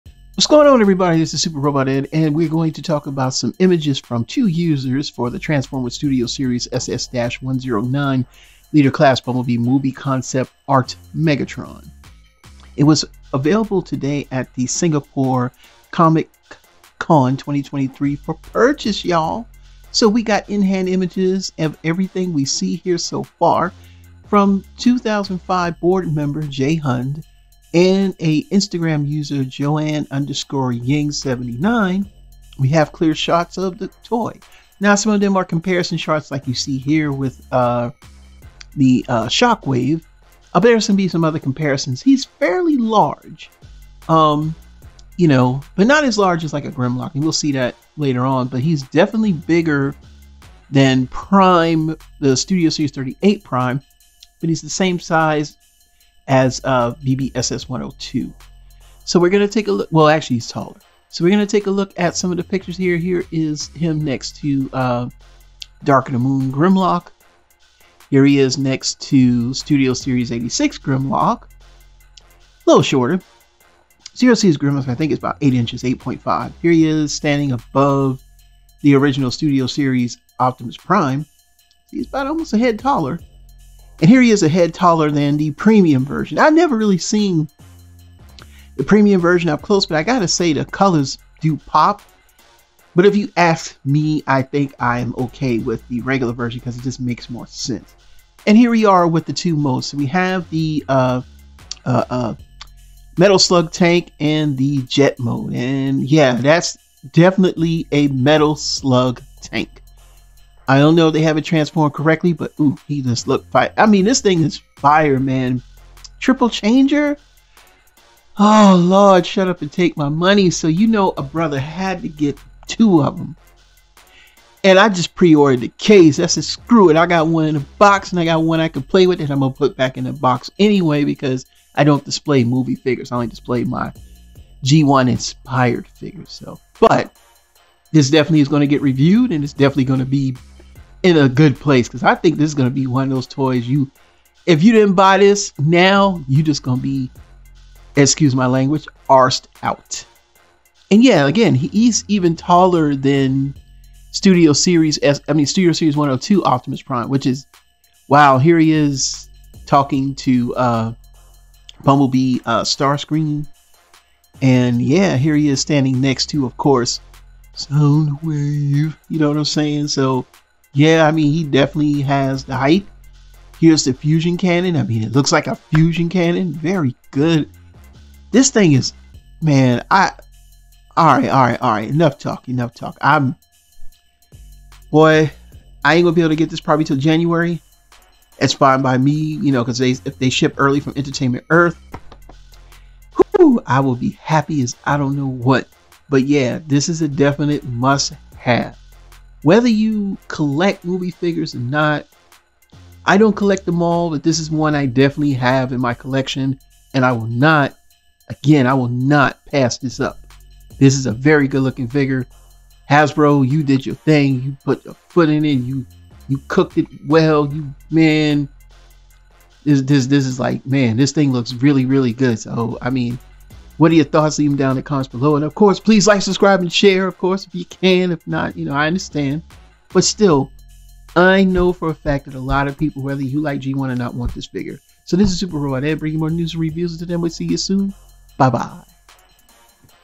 Transformers Studio Series Bumblebee Studio Concept Art Megatron In-Hand Pics @Singapore Comic Con!!
No One Under 13 Allowed!!